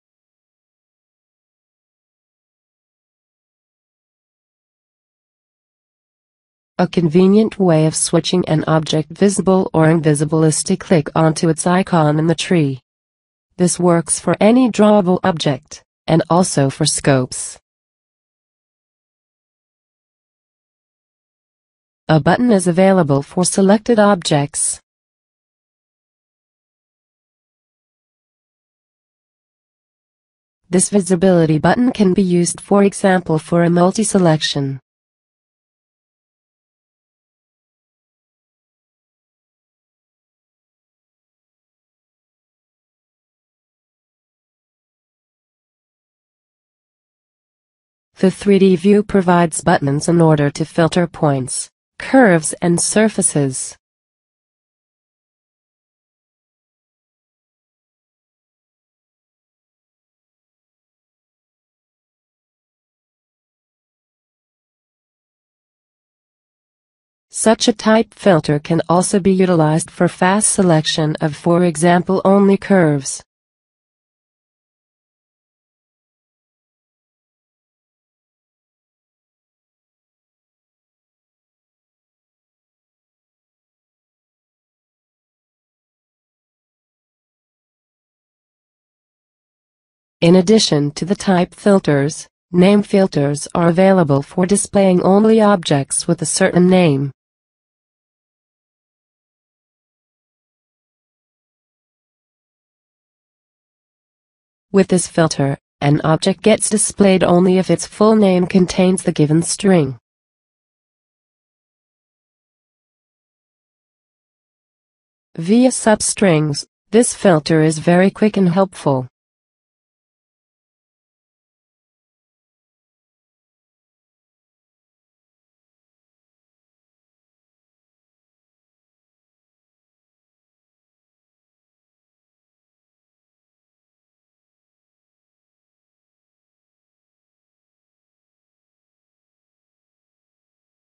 Visibility and Filters
This tutorial shows how to switch on and off the visibility of objects. In addition, the filter capabilities of the 3D view are demonstrated for exclusive views and smarter selections.